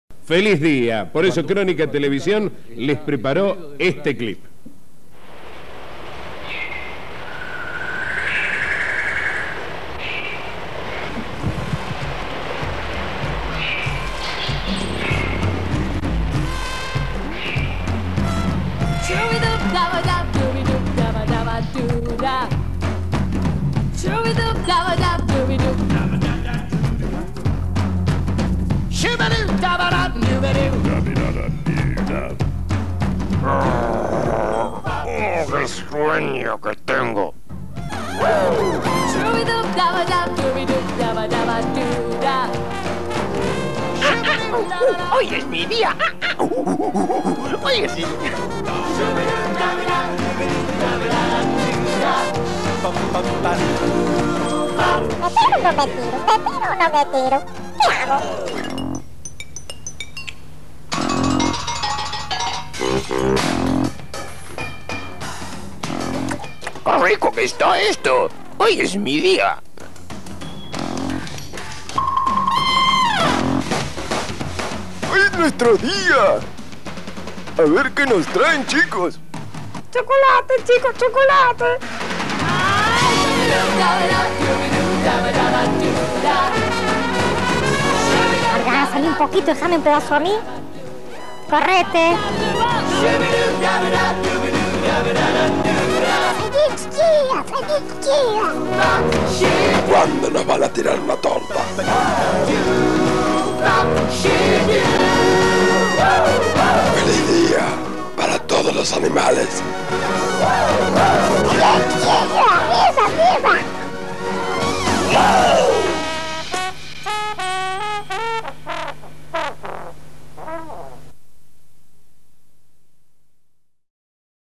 DÍA DEL ANIMAL (1998)
En Crónica TV solíamos hacer clips para fechas importantes, como día de la madre, padre, niño, animal, etc. Y a mí me encantaba molestar a mis compañeros de archivo buscando material entre cientos de notas, para poder hacerlos. Igualmente, los camarógrafos entre nota y nota, hacían imágenes para usar en esas oportunidades (si iban al zoo, o a alguna plaza). En éste se me ocurrió agregar las voces distorcionadas en off con algunos locutores que se coparon. Y encontrar la música no era tan fácil como ahora que existe Youtube. El musicalizador laburaba con discográficas, y siempre tenía material disponible para mí.
Cámaras: varios, especialmente Marcelo Dipalma
Musicalizador: Juan Devargo
Locucutor: Claudio Orellano presentando, Claudia Kaufman y  Javier Diaz las voces